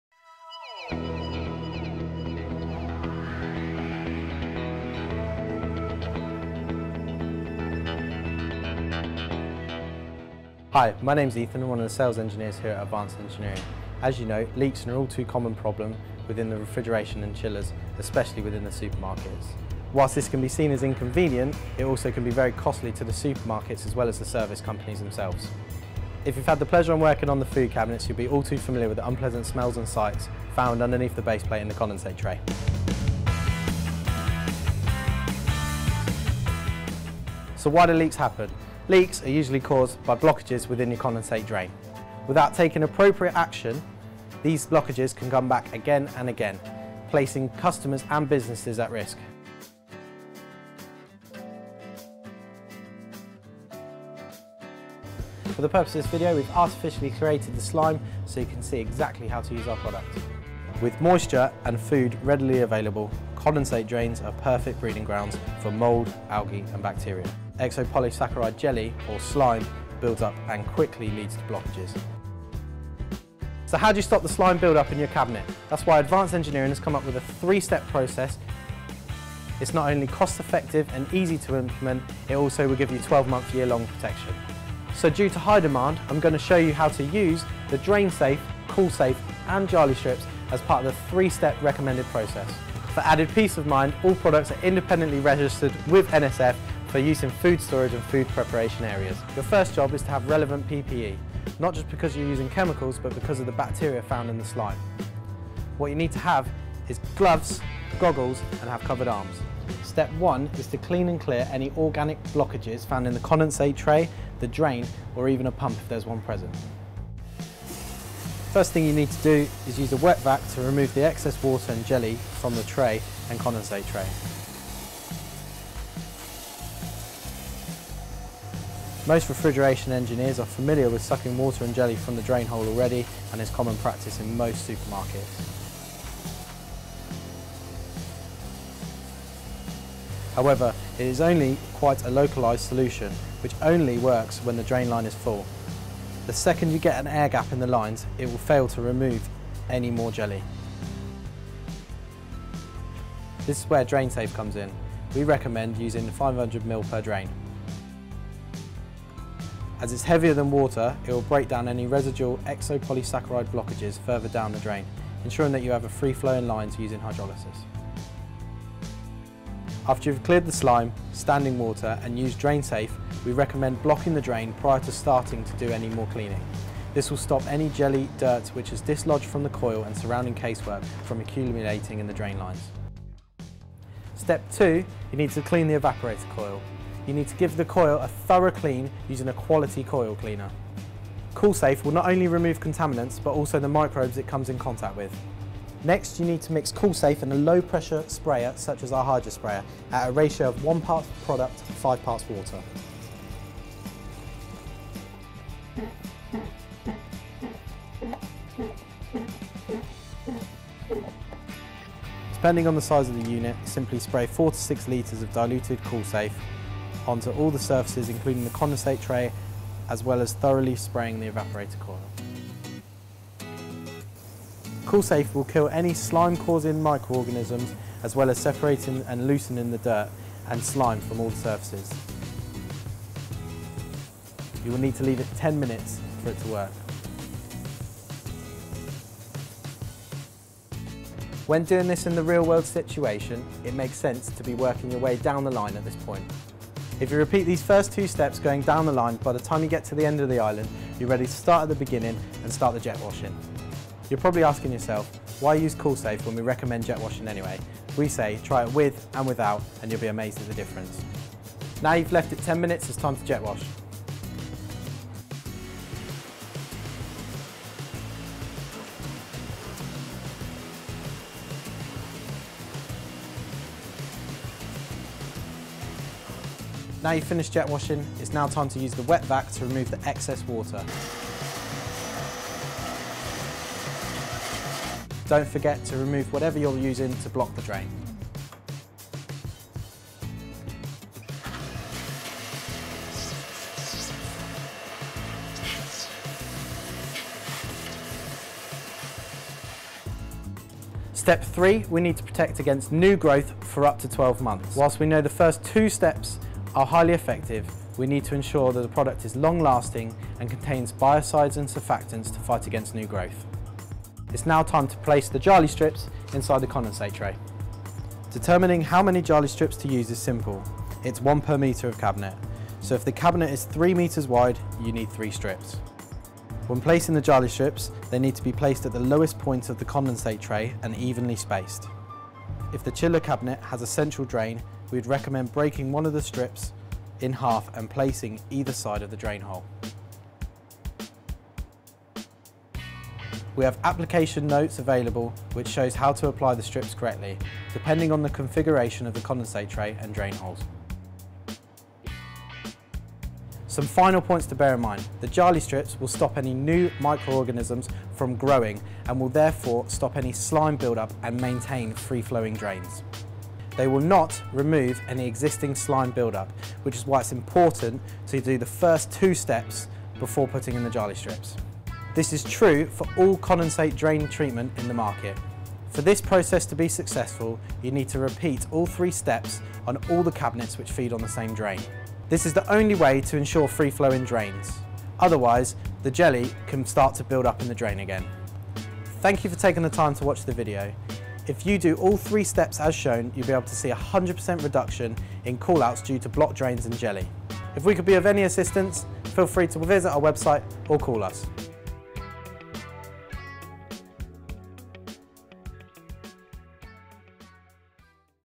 The Safe Range, three step solution, for preventing refrigeration drain blockages.
A quick video showing you have to clean and remove the micro-organisms and slime which caused refrigeration drain blockages, using the three-step Safe Range solution for 12 months protection.